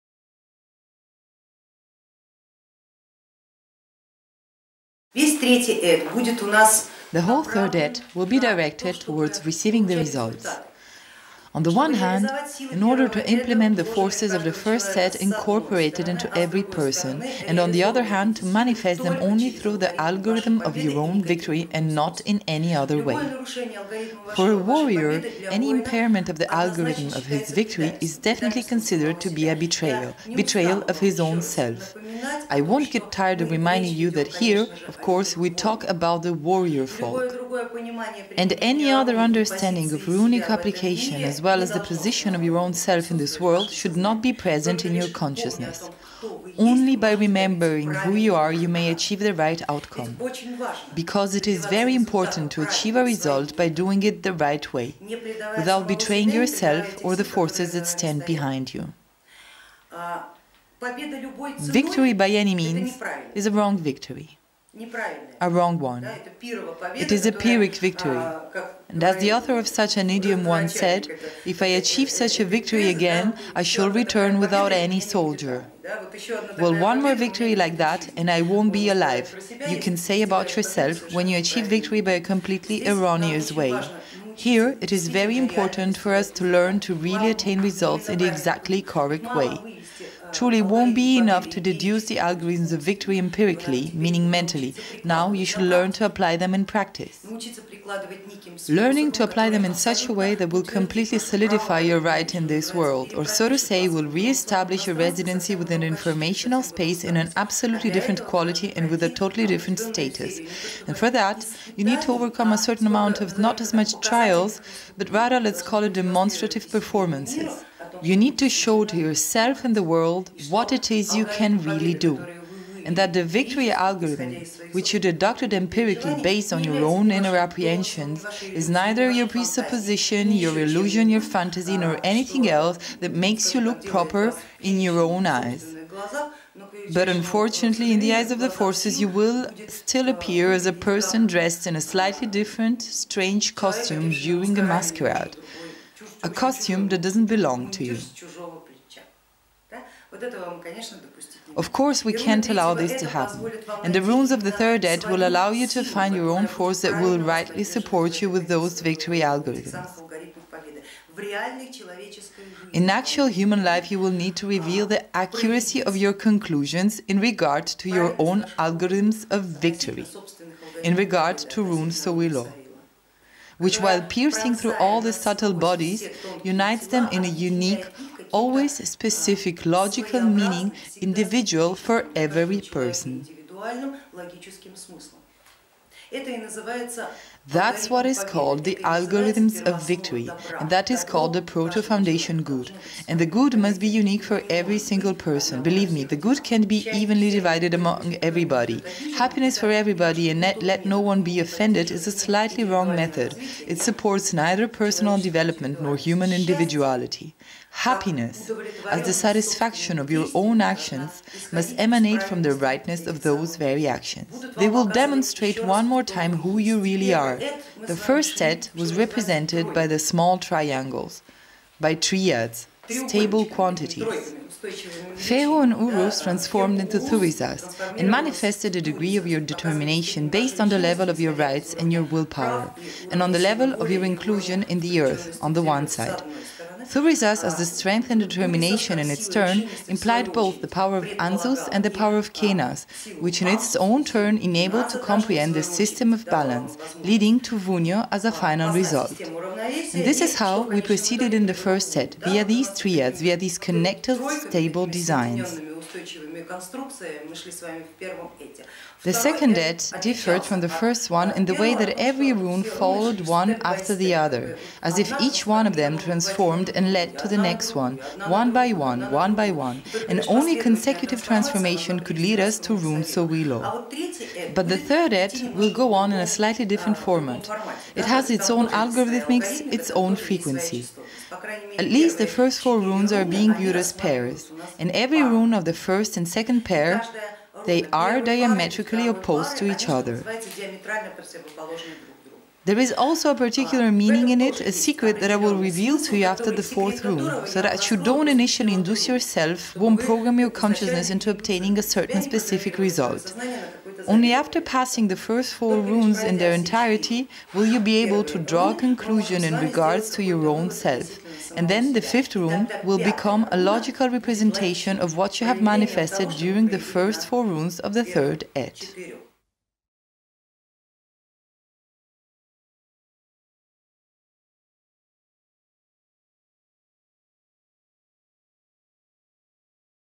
Runes Of The Third Aett Of The Elder Futhark. Excerpt From The Rune Course
The whole third aett will be directed towards receiving the results. On the one hand, in order to implement the forces of the first aett incorporated into every person. And on the other hand, to manifest them only through the algorithm of your own victory and not in any other way. For a warrior, any impairment of the algorithm of his victory is definitely considered to be a betrayal, betrayal of his own self. I won’t get tired of reminding you that here, of course, we talk about the warrior folk, and any other understanding of runic application as well as the position of your own self  in this world should not be present in your consciousness. Only by remembering who you are, you may achieve the right outcome. Because it is very important to achieve a result by doing it the right way, without betraying yourself or the forces that stand behind you. Victory by any means is a wrong victory, a wrong one. It is a pyrrhic victory, as the author of such an idiom once said, “if I achieve such a victory again, I shall return without any soldier”. Well, one more victory like that and I won’t be alive, you can say about yourself when you achieve victory by a completely erroneous way.